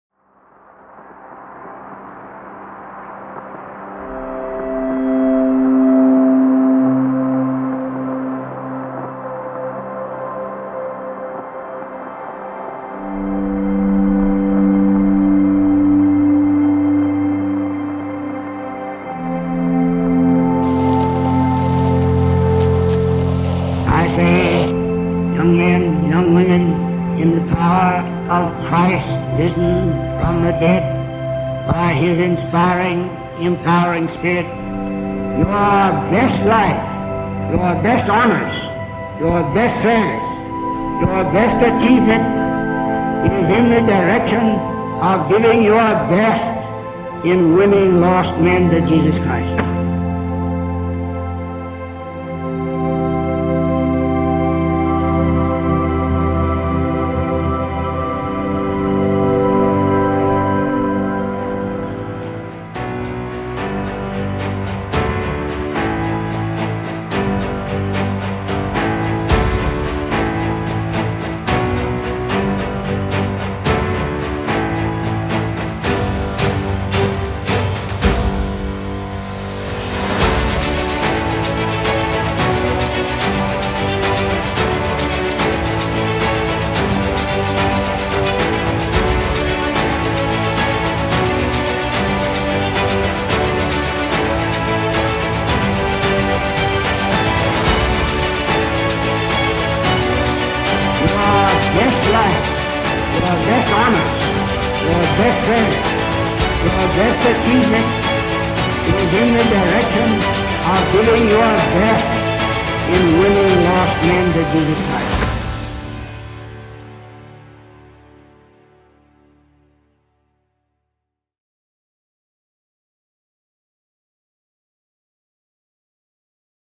Scarborough College - Launch Video
On April 12, 2017, trustees at Southwestern Baptist Theological Seminary announced a new name for the College at Southwestern. The college will be known as Scarborough College, taking it's name from L. R. Scarborough, the second president of Southwestern Baptist Theological Seminary.